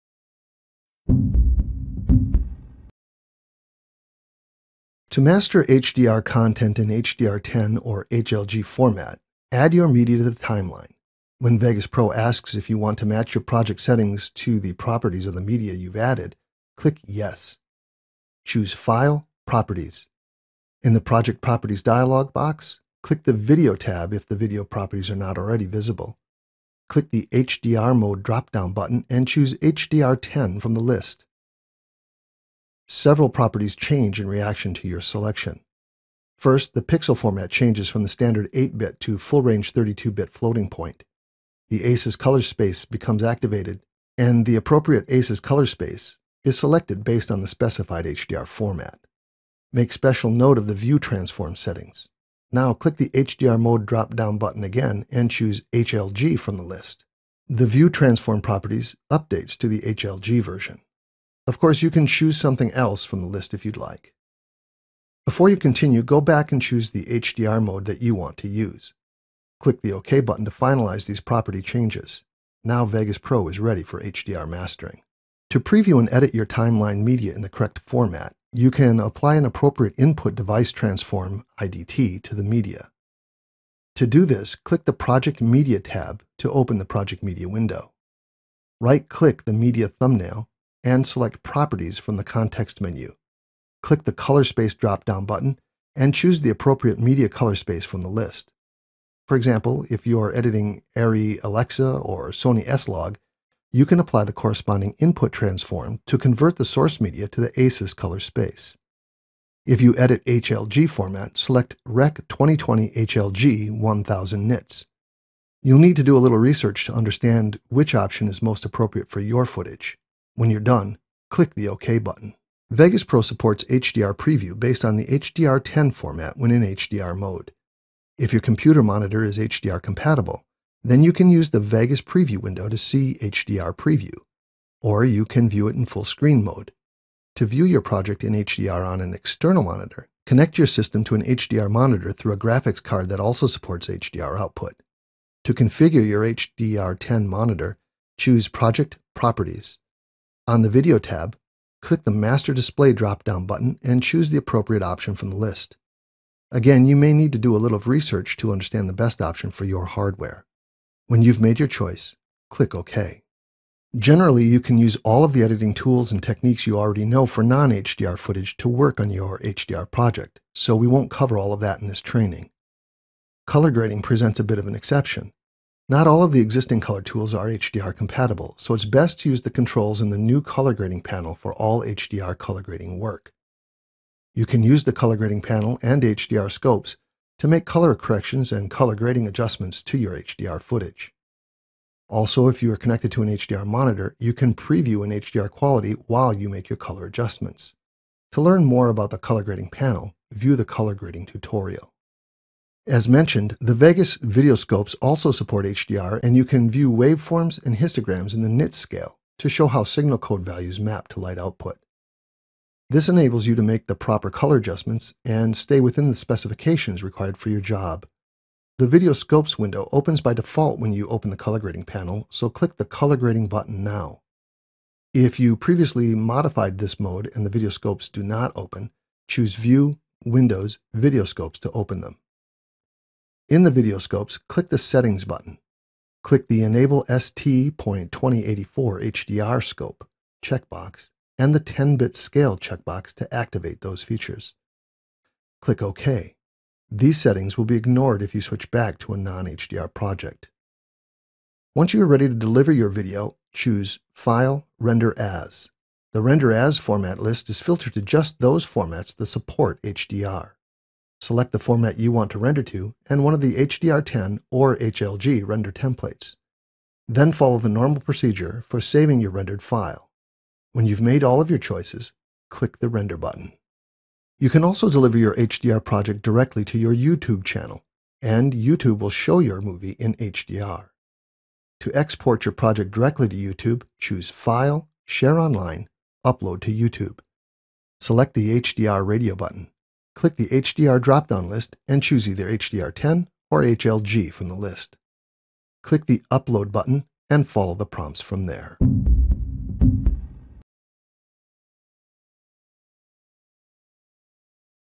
VEGAS Pro 17 - Complete HDR Color Support Tutorial
Get familiar with the HDR color support workflow in VEGAS Pro and learn how to preview, edit, and deliver your footage in full HDR glory.